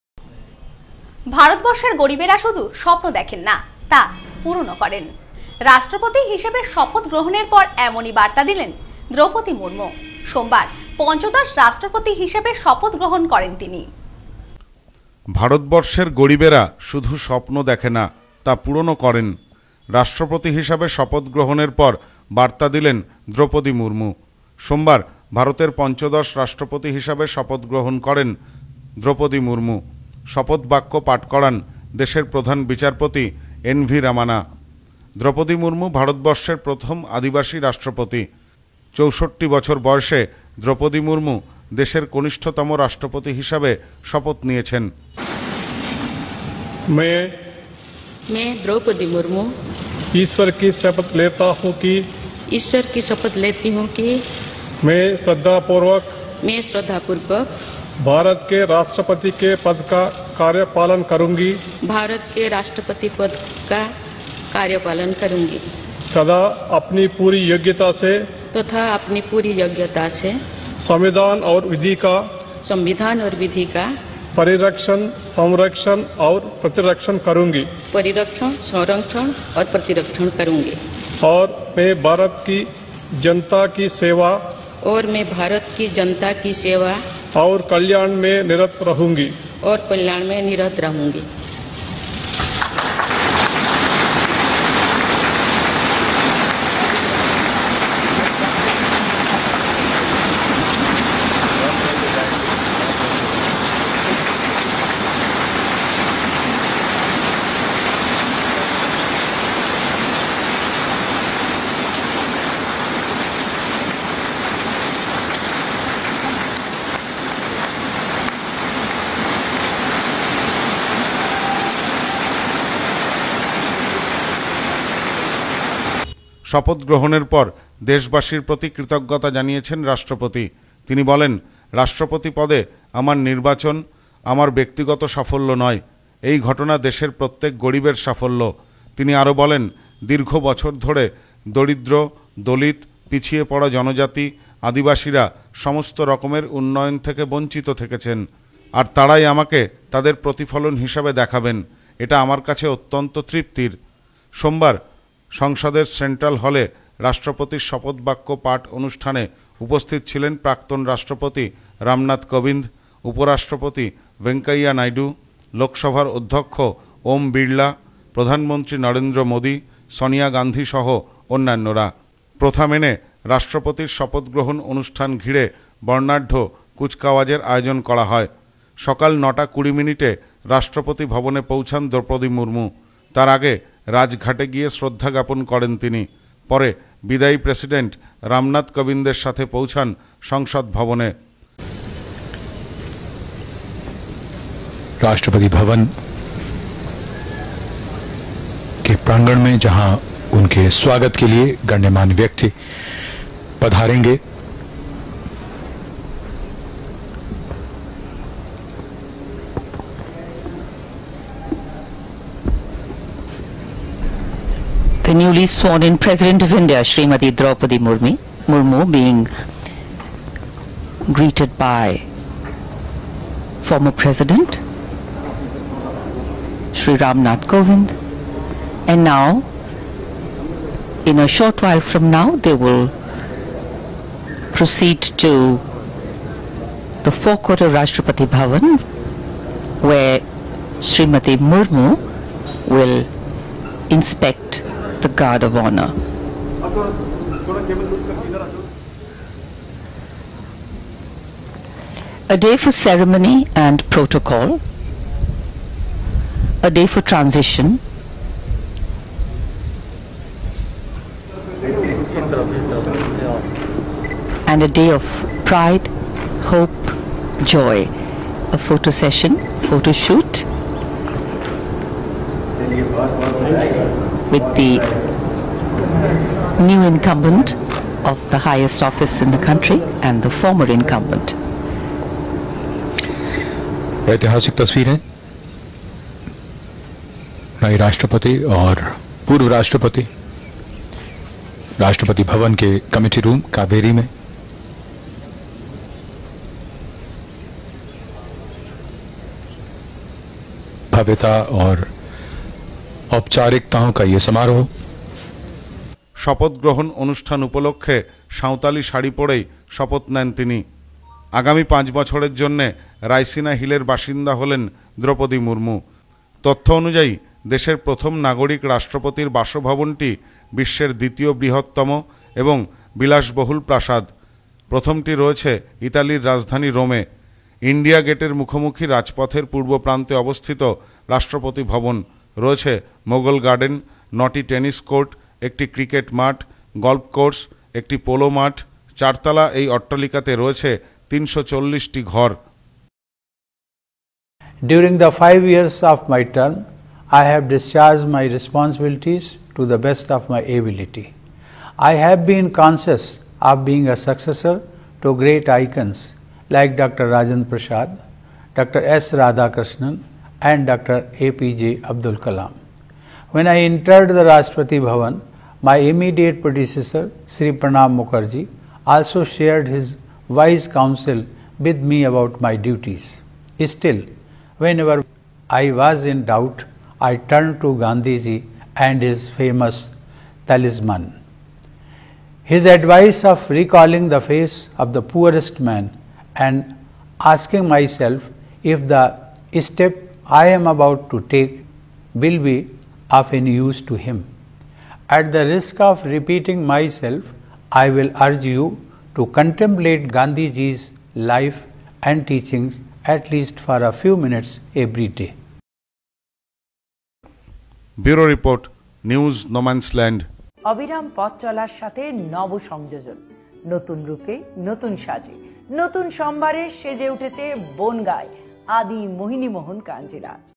সোমবার পঞ্চদশ রাষ্ট্রপতি হিসেবে শপথ নিলেন দ্রৌপদী মুর্মু ।
"ভারতবর্ষে গরিবেরা শুধু স্বপ্ন দেখেনা তা পূরণ করেন"রাষ্ট্রপতি হিসেবে শপথ গ্রহণের পর এমন বার্তা দিলেন দ্রৌপদী মুর্মু ।সোমবার পঞ্চদশ রাষ্ট্রপতি হিসেবে শপথ নেন তিনি।